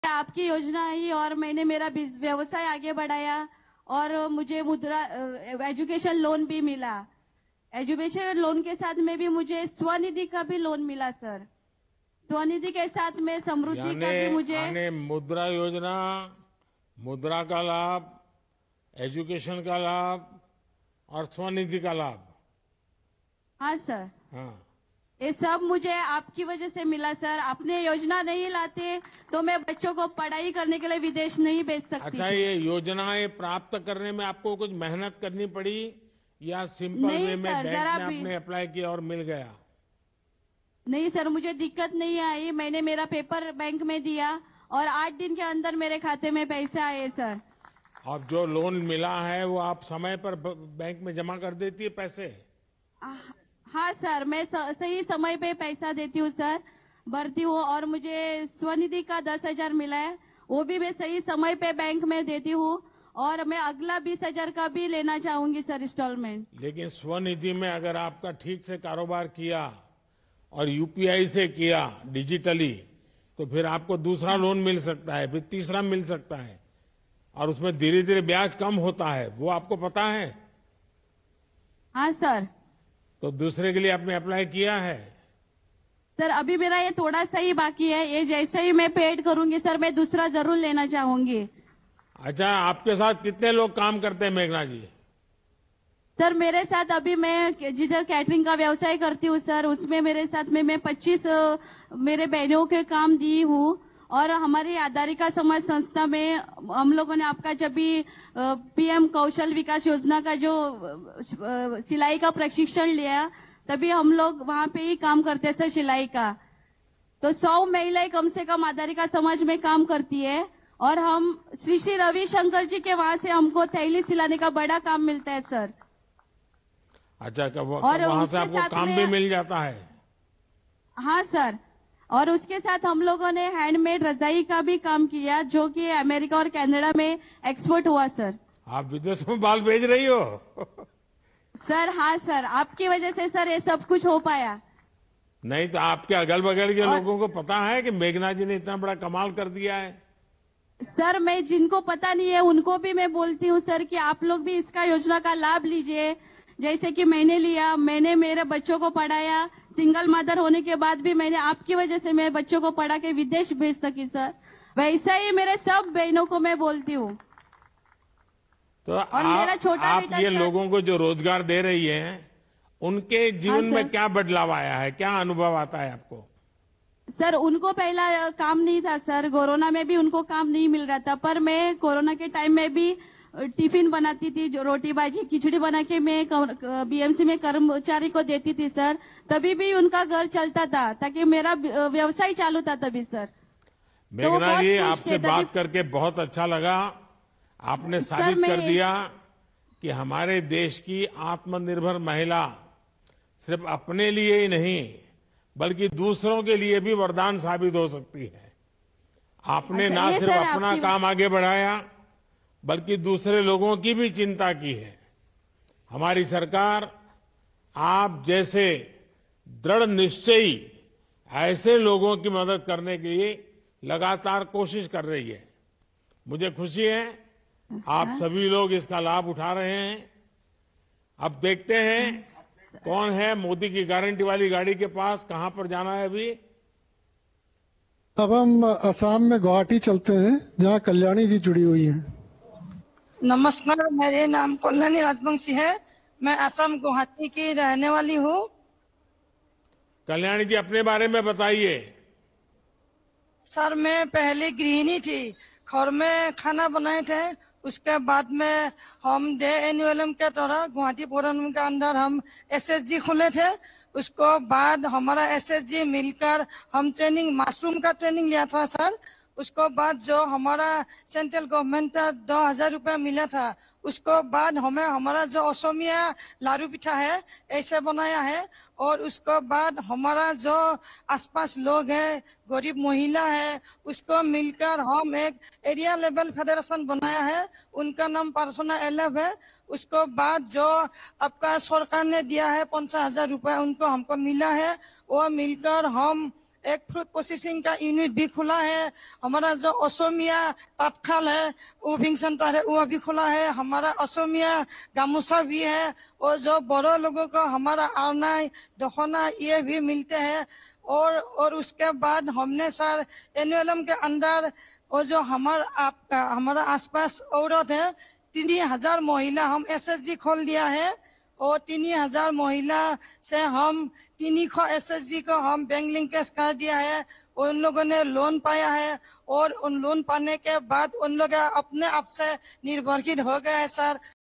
PM Modi ने विकसित भारत संकल्प यात्रा के लाभार्थियों से की बात
Viksit Bharat Sankalp Yatra: PM नरेंद्र मोदी ने विकसित भारत संकल्प यात्रा के लाभार्थियों से बात की. महिला लाभार्थियों ने योजनाओं को लेकर पीएम मोदी के साथ अपना अनुभव साज्ञा किया. पीएम ने भी उनके कार्यों को सहारा है.